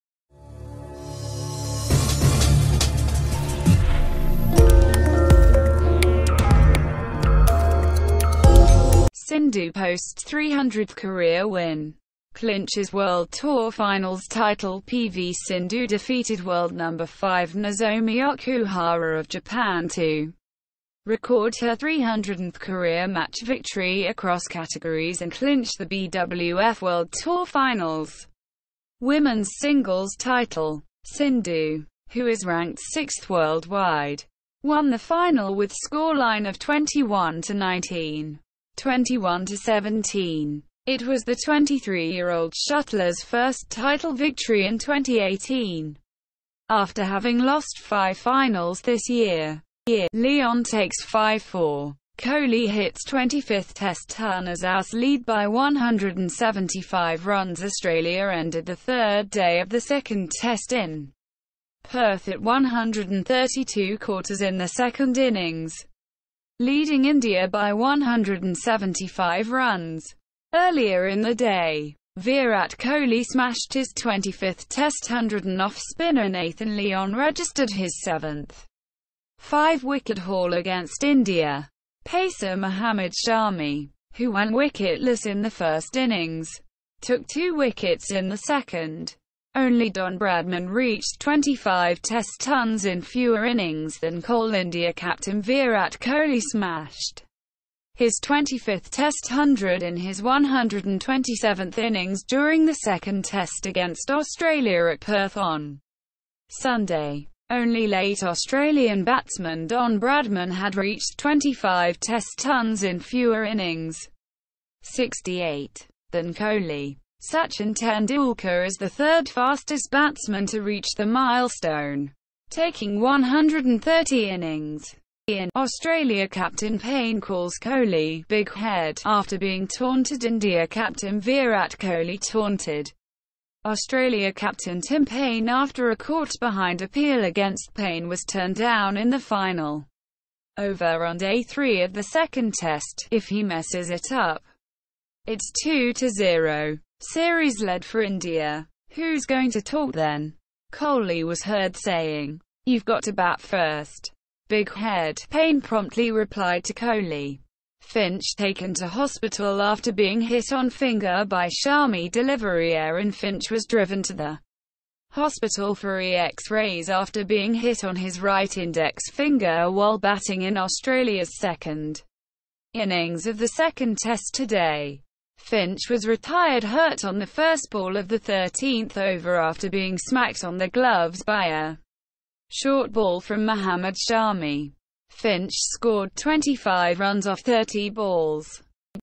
Latest Sports News - Sindhu posts 300th career win, clinches World Tour Finals title
--- Sindhu posts 300th career win, clinches World Tour Finals title ---
  PV Sindhu defeated world number five Nozomi Okuhara of Japan to record her 300th career match victory across categories and clinch the BWF World Tour Finals women's singles title. Sindhu, who is ranked sixth worldwide, won the final with scoreline of 21-19, 21-17. It was the 23-year-old shuttler's first title victory in 2018, after having lost five finals this year.
 --- Lyon takes 5-for, Kohli hits 25th Test ton as Aus lead by 175 runs ---
  Australia ended the third day of the second Test in Perth at 132/4 in the second innings, leading India by 175 runs. Earlier in the day, Virat Kohli smashed his 25th Test hundred and off-spinner Nathan Lyon registered his seventh five-wicket haul against India. Pacer Mohammad Shami, who went wicketless in the first innings, took two wickets in the second.
 --- Only Don Bradman reached 25 Test tons in fewer innings than Kohli ---
  India captain Virat Kohli smashed his 25th Test hundred in his 127th innings during the second Test against Australia at Perth on Sunday. Only late Australian batsman Don Bradman had reached 25 Test tons in fewer innings (68) than Kohli. Sachin Tendulkar is the third fastest batsman to reach the milestone, taking 130 innings.
 --- Australia captain Paine calls Kohli 'big head' after being taunted ---
  India captain Virat Kohli taunted Australia captain Tim Paine after a caught-behind appeal against Paine was turned down in the final over on day three of the second Test. "If he messes it up, it's 2-0 (series lead for India)...who's going to talk then?" Kohli was heard saying. "You've got to bat first, big head," Paine promptly replied to Kohli.
 --- Finch taken to hospital after being hit on finger by Shami delivery ---
  Aaron Finch was driven to the hospital for X-rays after being hit on his right index finger while batting in Australia's second innings of the second Test today. Finch was retired hurt on the first ball of the 13th over after being smacked on the gloves by a short ball from Mohammad Shami. Finch scored 25 runs off 30 balls.